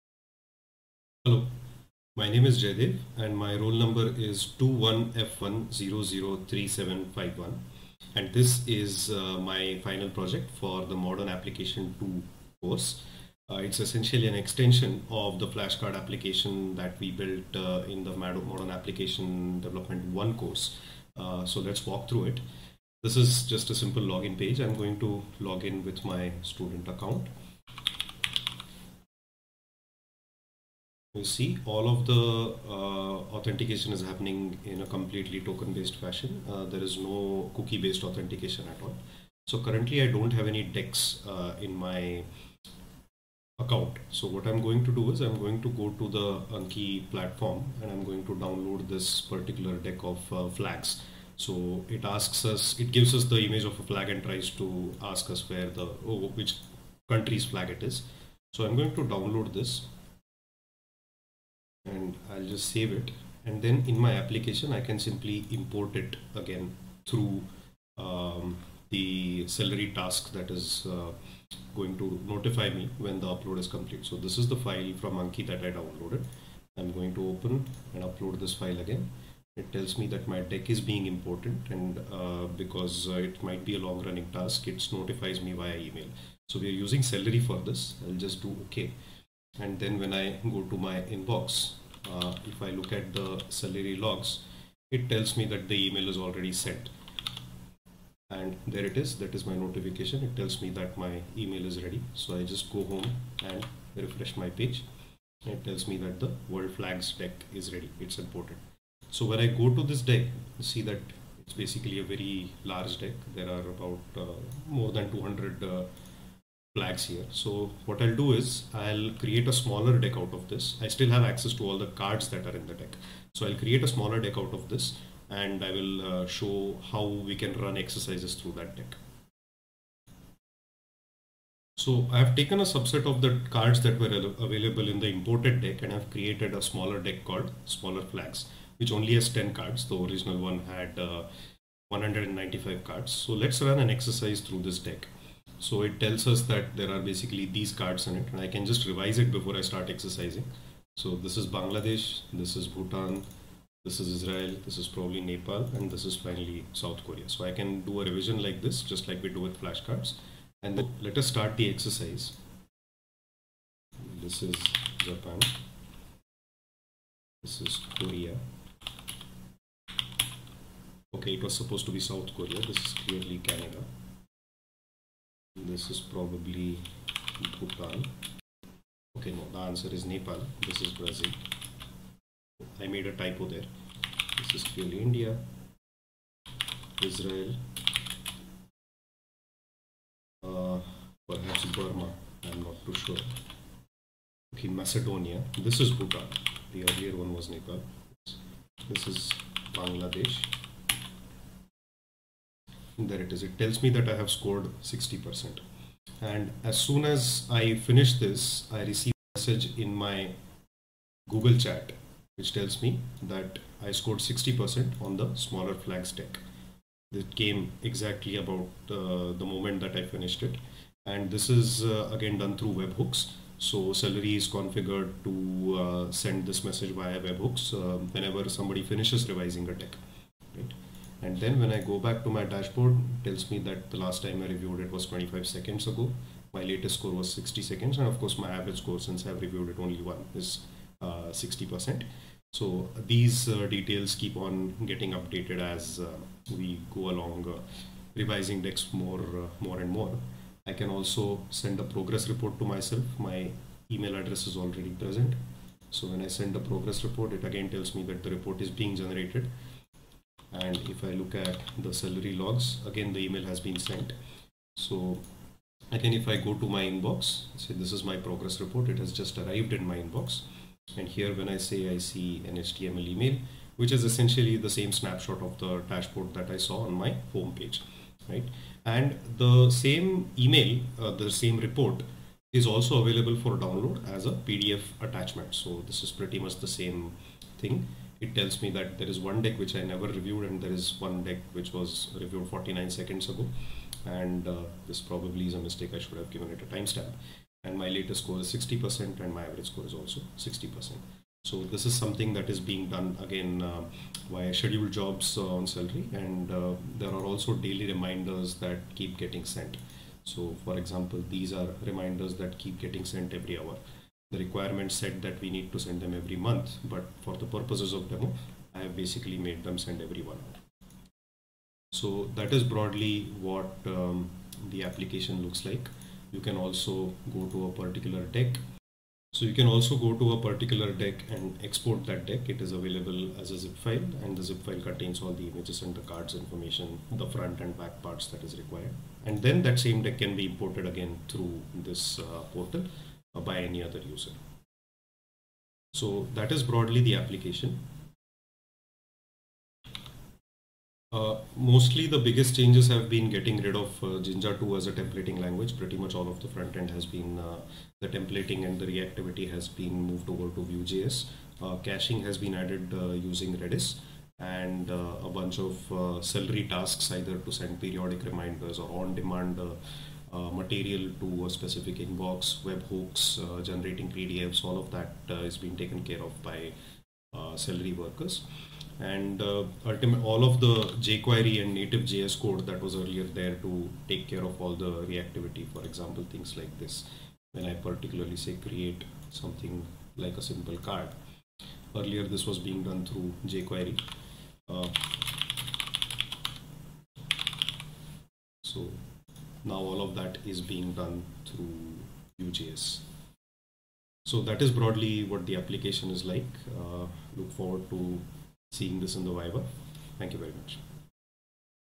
MAD2 Project Report
Video report for my project in the Modern Application Development - 2 course, offered as part of the data science diploma from IIT Madras Online Degree Program.

This project is flashcard app built with Vue.js and Flask, and makes heavy use of async tasks with Celery and webhooks.